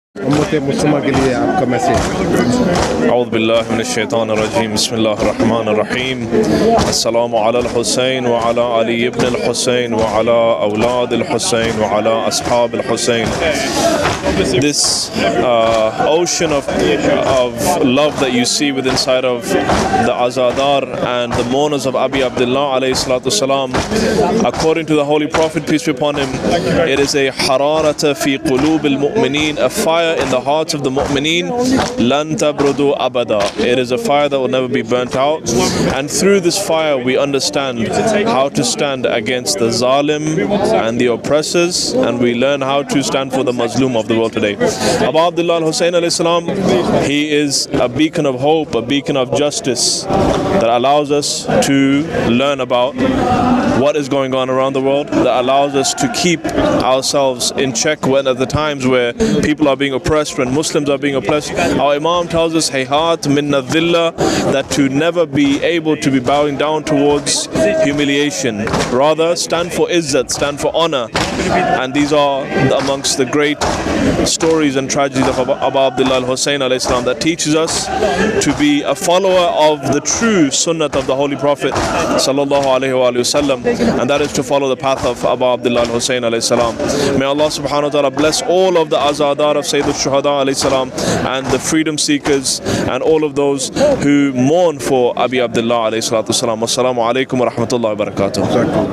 Allama Ahmed Ali Naqvi,message of Ashura. Manchester UK.
Day of Ashura, the 10th of Muharram,Imam Hussain (ra), grandson of Prophet Muhammad (SAAW), was martyred in the Battle of Karbala, along with his family and supporters by a so called tyrant ruler Yazid ibne Muawya Ummawai.

Imam Hussain (ra) had travelled to Iraq to answer the call of the Muslims of Kufa to come to their land so they could swear allegiance to him as their ruler.

But once he’d arrived at Karbala (modern-day Iraq), Imam Hussain (AS) was met by violent and torturous soldiers. In the swelteringly hot desert, Imam Hussain (AS) and his children were deprived of water for days, so that Imam Hussain (AS) was martyred, thre days thirsty. It’s narrated that his youngest child, at just six months old, killed by a three heads arrow shot at him when Imam Hussain AS asked a sip of water for the him during this battle.